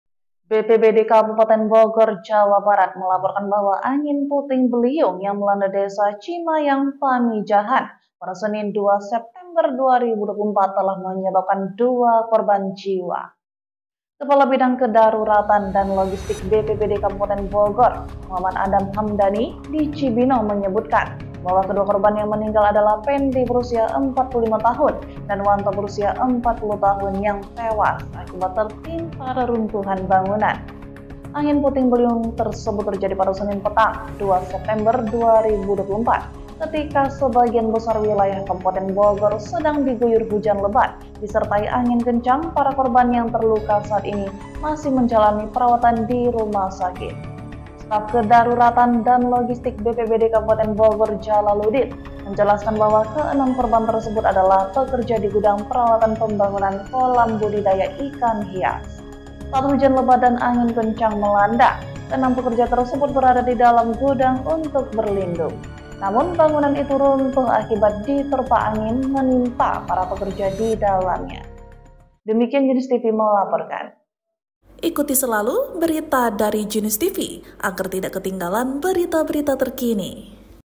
Angin Puting Beliung di Bogor, Dua Orang Meninggal Dunia
Bencana angin puting beliung melanda Desa Cimayang, Pamijahan, Kabupaten Bogor pada Senin petang (2/9/2024), mengakibatkan dua korban jiwa dan empat lainnya terluka. Badan Penanggulangan Bencana Daerah (BPBD) Kabupaten Bogor melaporkan bahwa kedua korban tewas, Pendi (45) dan Wanto (40), meninggal dunia setelah tertimpa reruntuhan bangunan saat berusaha berlindung dari hujan lebat dan angin kencang.

Empat korban lainnya yang terluka—Suyoto, Iman Gojali, Tofik, dan Wiwaluyo—kini tengah menjalani perawatan di RSUD Leuwiliang. Menurut staf Kedaruratan dan Logistik BPBD, Jalaludin, keenam korban adalah pekerja di gudang peralatan pembangunan kolam budidaya ikan hias, yang runtuh diterpa angin puting beliung.

Saksikan detail lengkapnya serta update terbaru dari kejadian ini di channel kami.